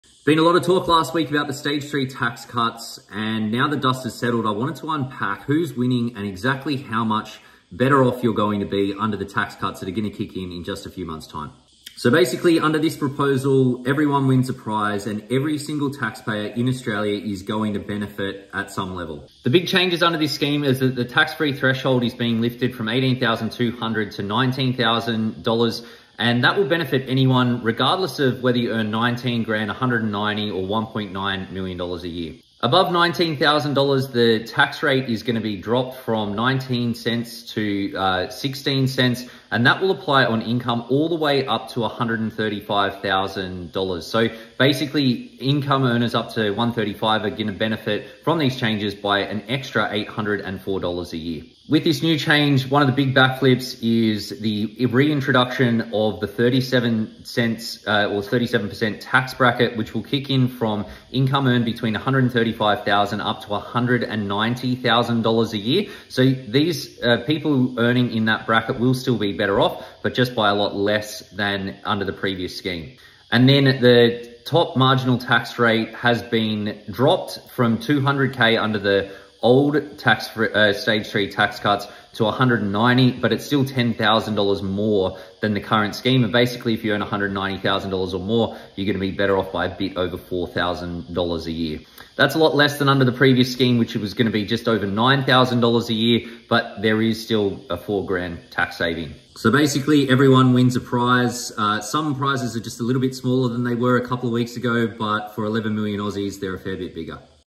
How much tax you’ll save from 1 July
New taxcuts are kicking in soon, here I cover how much you’ll get and the taxsavings under the new changes